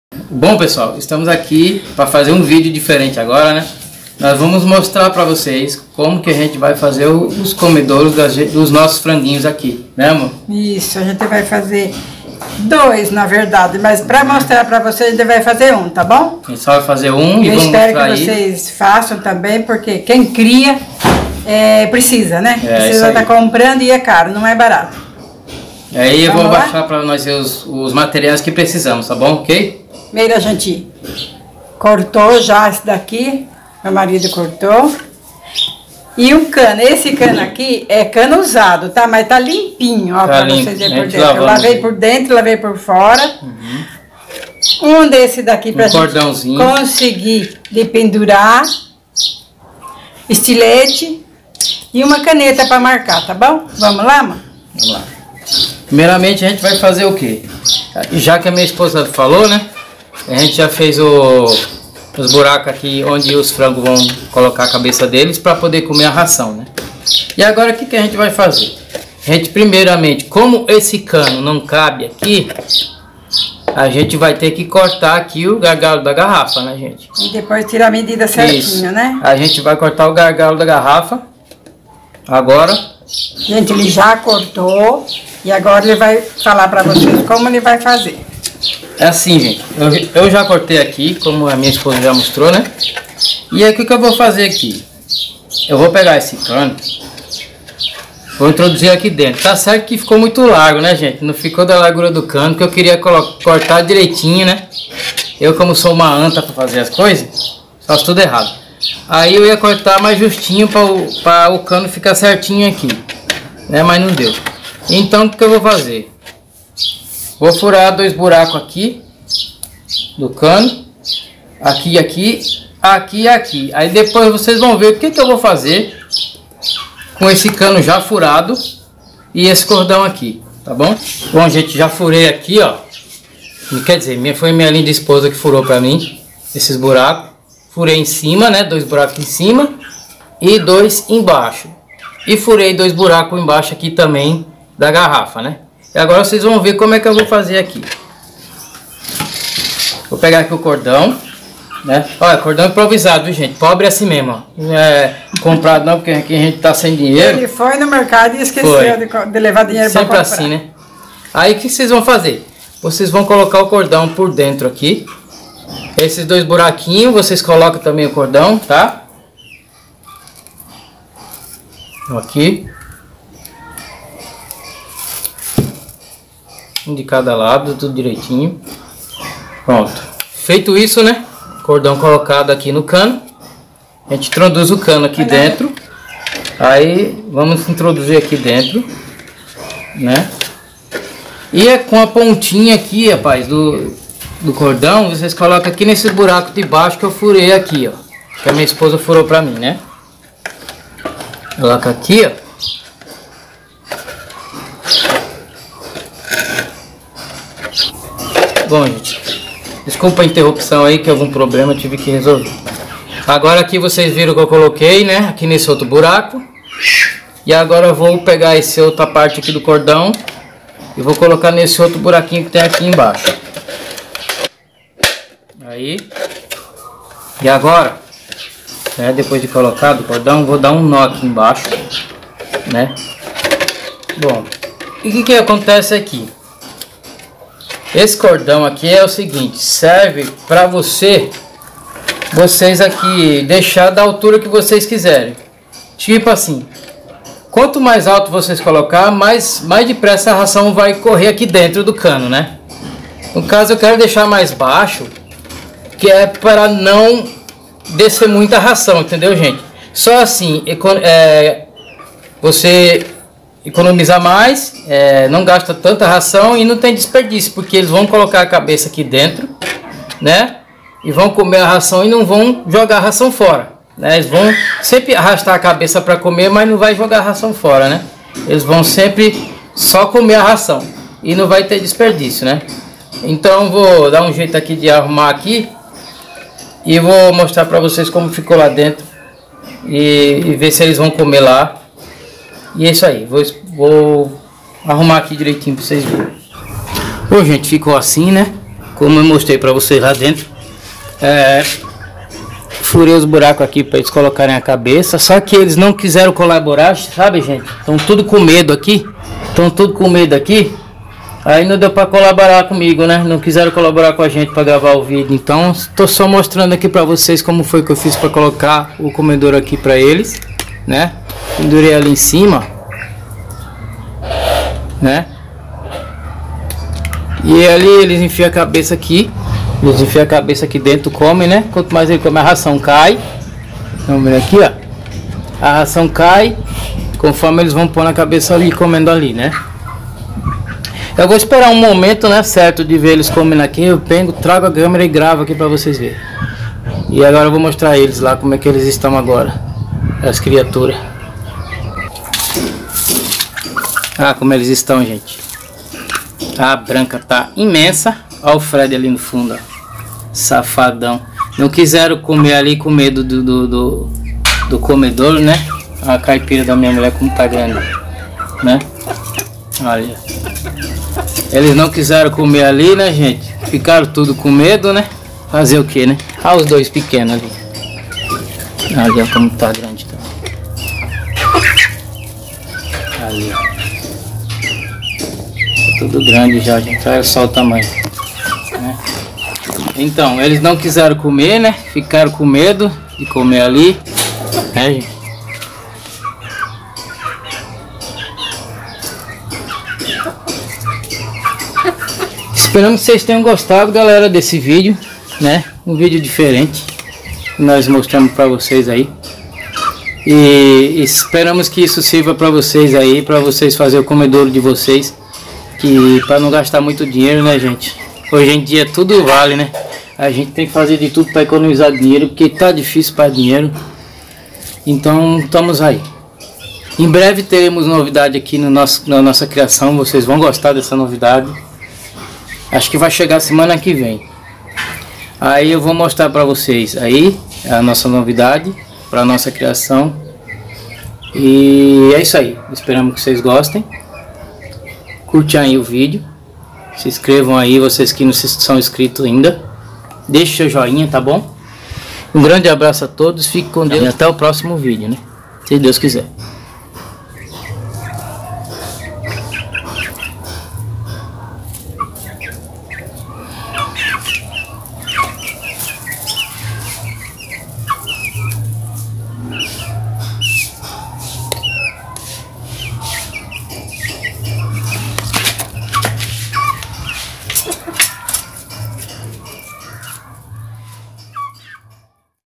FAÇA UM COMEDOURO SIMPLES E BARATO...VALE A PENA
Neste vídeo mostraremos como fazer um comedouro simples e barato